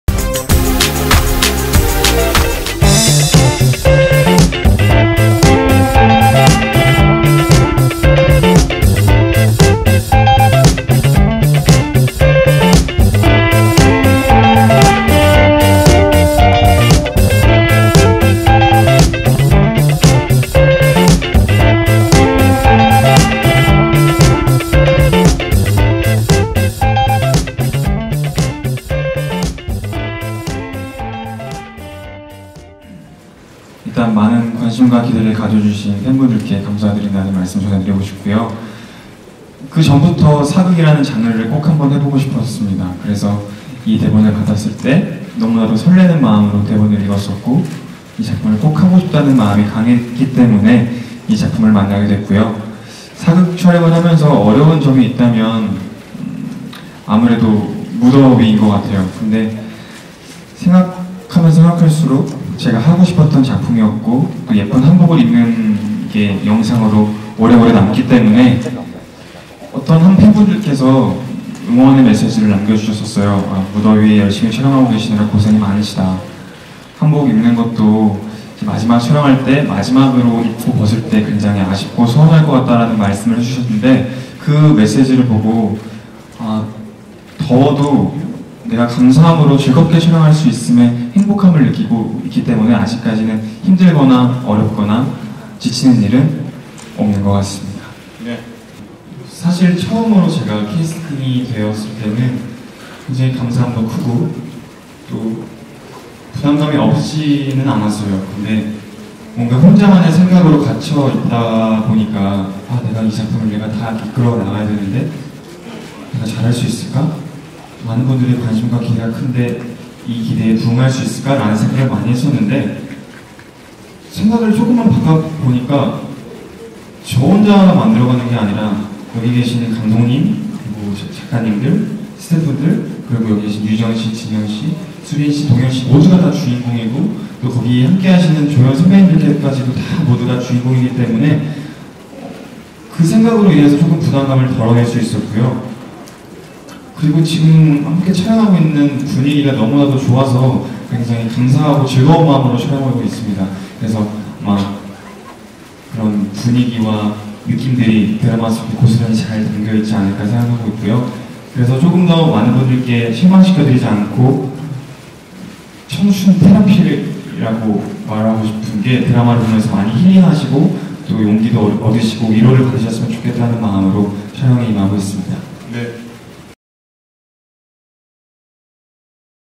[TV10]왕이 되고 싶었던 박보검!
[TV10]박보검의 첫 사극 주연작 '구르미 그린 달빛' 제작발표회